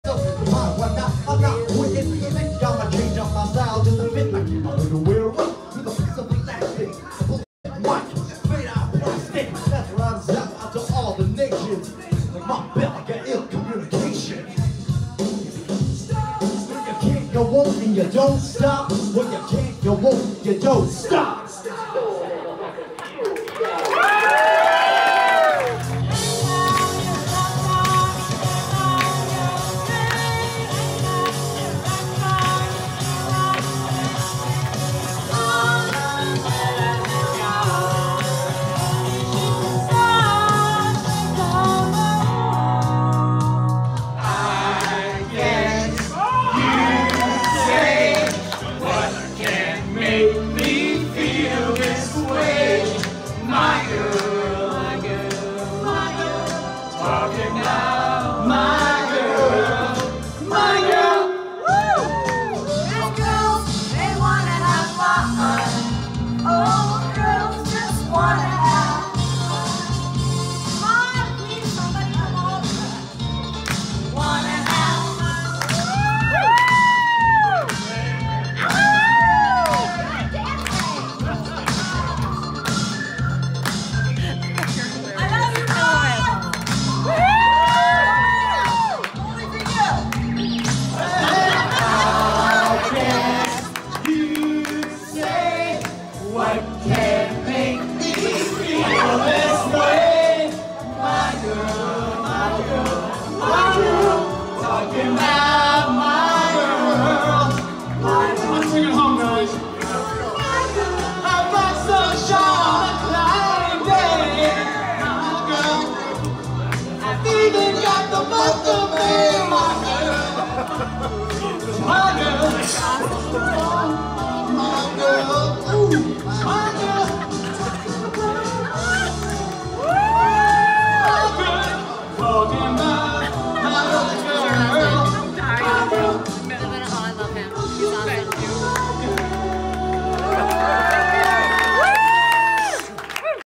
Karaoke Idol 12 9 17
Highlights from my Milestone Birthday Bash at Harry Caray's Rosemont - Karaoke Idol!!!